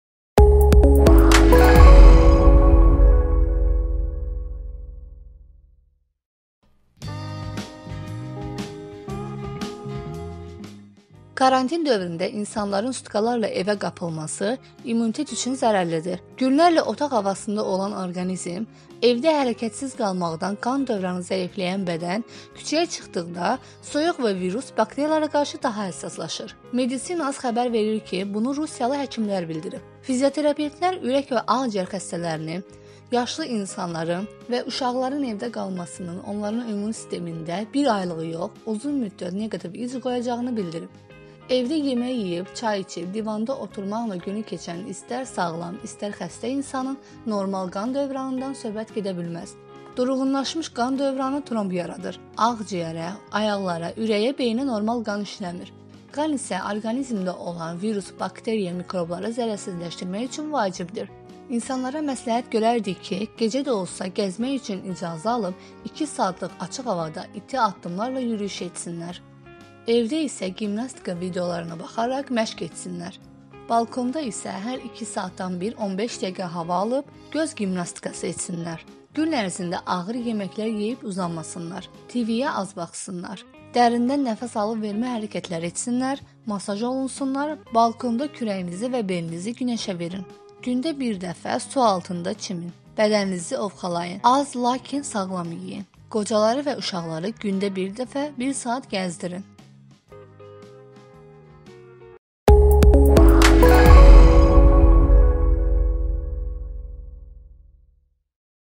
Həkimlər açıqladı: Evə qapanmaq immuniteti çökdürə bilər
Kanal Azərbaycanın ən öndə gedən tibb portalına - Medicina.az-a məxsusdur.

Karantin dövründə insanların sutkalarla evə qapadılması immunitet üçün zərərlidir. Günlərlə otaq havasında olan orqanizm, evdə hərəkətsiz qalmaqdan qan dövranı zəifləyən bədən küçəyə çıxdıqda soyuq və virus, bakteriyalara qarşı daha həssaslaşır.

Medicina.az xəbər verir ki, bunu rusiyalı həkimlər bildirib. Fizioterapevtlər ürək və ağciyər xəstələrinin, yaşlı insanların və uşaqların evdə qalmasının onların immun sistemində 1 aylıq yox, uzun müddət neqativ iz qoyacağını bildirir.

“Evdə yemək yeyib,çay içib, divanda oturmaqla günü keçən istər sağlam, istər xəstə insanın normal qan dövranından söhbət gedə bilməz. Durğunlaşmış qan dövranı tromb yaradır, ağciyərə, ayaqlara, ürəyə, beyinə normal qan işləmir. Qan isə orqanizmdə olan virus, bakteriya, mikrobları zərərsizləşdirmək üçün vacibdir.
İnsanlara məsləhət görərdik ki, gecə də olsa gəzmək üçün icazə alıb 2 saat açıq havada iti addımlarla yürüş etsinlər. Evdə isə gimnastika videolarına baxaraq, məşğələlər etsinlər. Balkonda çıxıb hər 2 saatdan bir 15 dəqiqə hava alıb, göz gimnastikası etsinlər. Gün ərzində ağır yeməklər yeyib uzanmasınlar.

TV-ə az baxsınlar. Dərindən nəfəs alıb vermə hərəkəti etsinlər. Masaj olunsunlar. Balkonda kürəyinizi və belinizi günəşə verin. Gündə bir dəfə kontast su altında çimin, bədəninizi ovxalayın. Az, lakin sağlam yeyin. Qocaları və uşaqları gündə 1 dəfə1 saat gəzdirin”